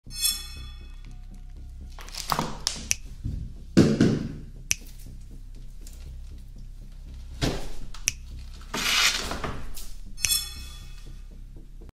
Ferrari Pumps vs. Enchanted Peaches! Heels Crushing Food! Oddly Satisfying ASMR
Welcome to Crushing Land, where food thinks and Pea's shoes engage in oddly satisfying battles! In today’s video, join Pea as she is crushing the enchanted Peaches, absorbing their natural energy with her Ferrari Pumps! This special training, not only enhances her awareness but also fosters her peaceful warrior mindset! Be sure to vote on the channel community tab if you want to enjoy the full version in the next episode!